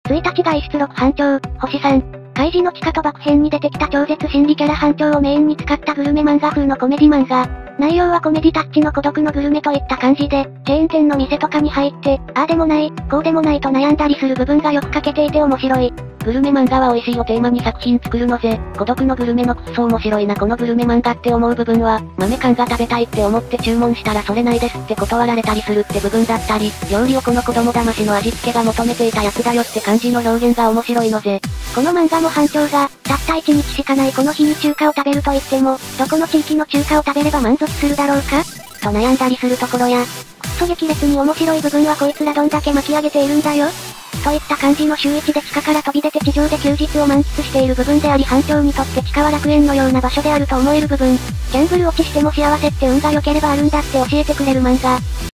【★★★】1日外出録ハンチョウ / 福本伸行 萩原天晴 上原求 新井和也 このマンガがすごい！2018 オトコ編 8位【死ぬ前にコレを見ろ レビュー あらすじ】
0:00 【あらすじ レビュー】
1日外出録ハンチョウ

このマンガがすごい！2018 オトコ編 8位

カイジの地下賭博編に出てきた超絶心理キャラ
班長をメインに使ったグルメマンガ風のコメディ
マンガ

内容はコメディタッチの孤独のグルメといった
感じで、チェーン店の店とかに入って、あーでも
ない、こーでもないと悩んだりする部分が良く
描けていて面白い

グルメマンガは美味しいをテーマに作品作るのぜ、
孤独のグルメのクッソ面白いなこのグルメマン
ガって思う部分は、豆缶が食べたいって思って
注文したらそれ無いですって断られたりするって
部分だったり、料理をこの子供だましの味付けが
求めていたやつだよ！って感じの表現が面白いのぜ

この漫画も班長が、たった一日しかないこの日に
中華を食べると言っても、どこの地域の中華を食べ
れば満足するだろうか？と悩んだりする所や

クッソ激烈に面白い部分はコイツらどんだけ巻き上げて
いるんだよ？っといった感じの修一で地下から飛び出て
地上で休日を満喫している部分であり班長にとって
地下は楽園のような場所であると思える部分

ギャンブル堕ちしても幸せって運が良ければあるんだ
って教えてくれる漫画

福本伸行 萩原天晴 上原求 新井和也
【注意 - NOTE】
Let's play !! - ゲーム実況